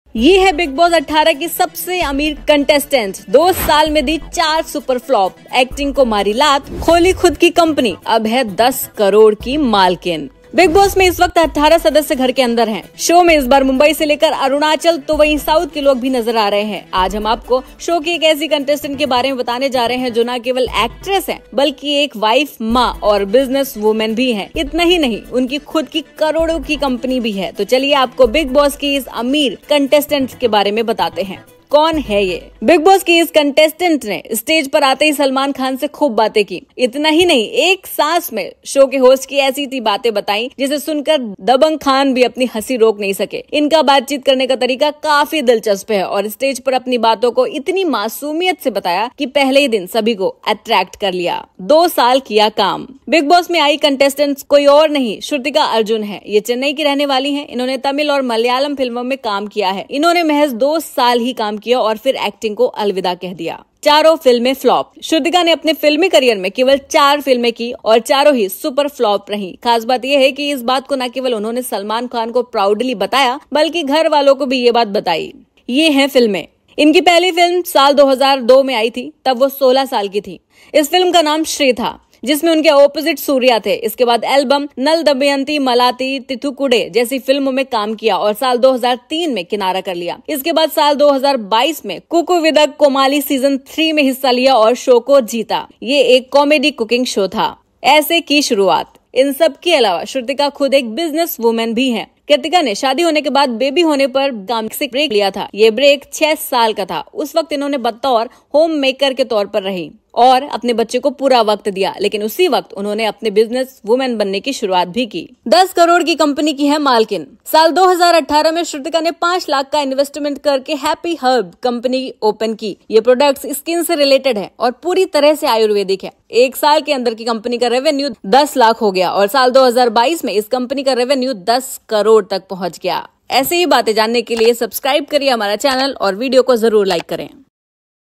Bigg Boss 18 की सबसे अमीर कंटेस्टेंट अब हैं 10 करोड़ की मालकिन | Shrutika Arjun
Bigg Boss 18 की सबसे अमीर कंटेस्टेंट, 2 साल में दी 4 सुपरफ्लॉप, एक्टिंग को मारी लात, खोली खुद की कंपनी, अब हैं 10 करोड़ की मालकिन | Shrutika Arjun |